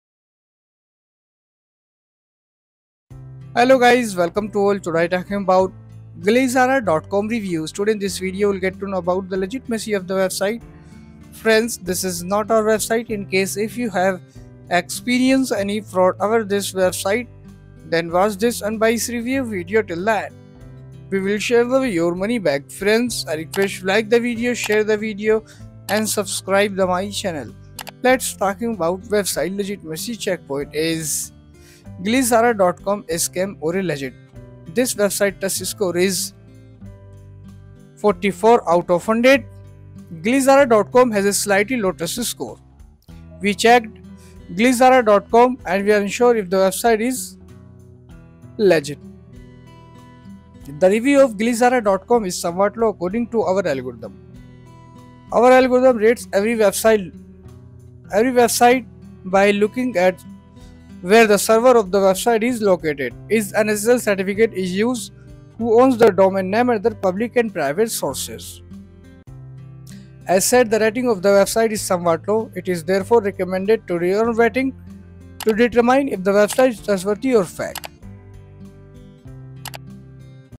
Glizara Reviews (April 2024) - Is This A Legit E-Commerce Site? Find Out! | Product Review
Do you want to know about Glizara Reviews? Watch this video till end because in this video you will get to know about the legitimacy of the website. Friends, this is not our website. In case, If you have experienced any fraud over this website then watch this unbiased review video till the end, We will share the way to get your money back.

Product Review